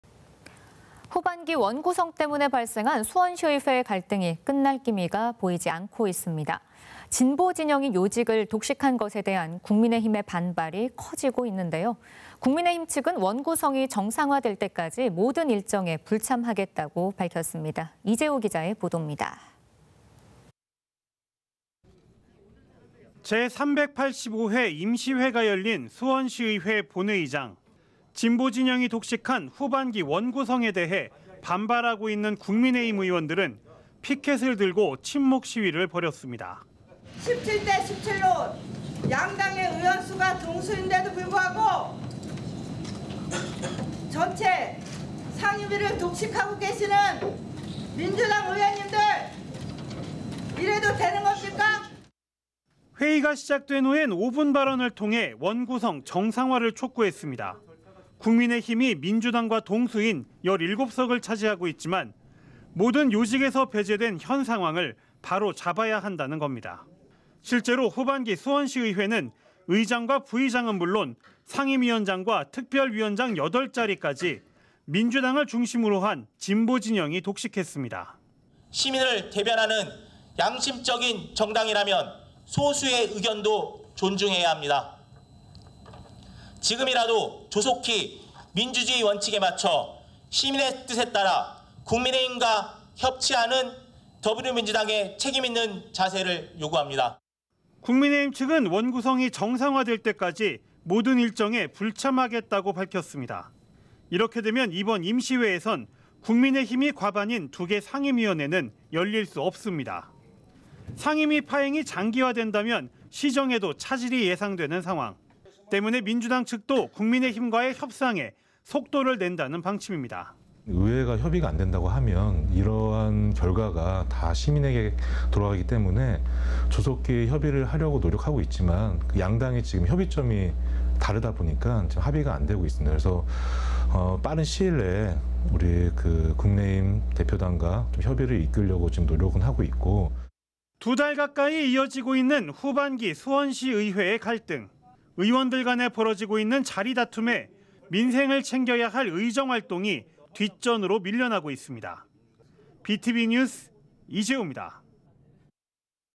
"민주당 독주 멈춰라"...수원시의회 결국 '파행'
[앵커멘트]

후반기 원 구성 때문에 발생한 수원시의회의 갈등이
끝날 기미가 보이지 않고 있습니다.

진보 진영이 요직을 독식한 것에 대한
국민의힘의 반발이 커지고 있는데요.

국민의힘 측은 원 구성이 정상화될 때까지
모든 일정에 불참하겠다고 밝혔습니다.

이재호 기자의 보도입니다.

[리포트]

제385회 임시회가 열린 수원시의회 본회의장.

진보 진영이 독식한 후반기 원 구성에 대해
반발하고 있는 국민의힘 의원들은
피켓을 들고 침묵 시위를 벌였습니다.

[유준숙 / 수원시의회 국민의힘 대표 의원]
"17대 17로 양당의 의석 수가 동수인데도 불구하고
전체 상임위를 독식하고 계신 민주당 의원님들.이래도 되는 것입니까?"

회의가 시작된 후에는 5분 발언을 통해
원 구성 정상화를 촉구했습니다.

국민의힘이 민주당과 동수인
17석을 차지하고 있지만
모든 요직에서 배제된 현 상황을
바로잡아야 한다는 것입니다.

실제로 후반기 수원시의회는
의장과 부의장은 물론
상임위원장과 특별위원장 8자리까지
민주당을 중심으로 한 진보 진영이 독식했습니다.

[홍종철 / 수원시의회 국민의힘 의원]
"시민을 대변하는 양심적인 정당이라면
소수의 의견도 존중해야합니다.
지금이라도 조속히 민주주의 원칙에 맞춰
시민의 뜻에 따라 국민의힘과 협치하는
더불어민주당의 책임있는 자세를 요구합니다."

이렇게 되면 이번 임시회에서는
국민의힘이 과반이 2개 상임위원회는
열릴 수 없습니다.

상임위 파행이 장기화된다면
시정에도 차질이 예상되는 상황.

때문에 민주당 측도 국민의힘과의 협상에
속도를 낸다는 방침입니다.

[김동은 / 수원시의회 민주당 대표 의원]
"의회가 협의가 안 된다고 하면
이러한 결과가 다 시민에게 돌아가기 때문에
조속히 협의를 하려고 노력하고 있지만
양당의 협의점이 다르다보니 합의가 안 되고 있습니다.
그래도 빠른 시일 내에 국민의힘 대표단과
협의를 하려고 노력을 하고 있고."

두 달 가까이 이어지고 있는
후반기 수원시의회의 갈등.

의원들 간에 벌어지고 있는 자리 다툼에
민생을 챙겨야할 의정 활동이
뒷전으로 밀려나고 있습니다.

[영상취재 최진]

■ 뉴스 시청 및 제보
▷ 케이블TV : SK브로드밴드 B tv 채널 1번
▷ 유튜브 : 'ch B tv' 검색 (SK브로드밴드 수원방송)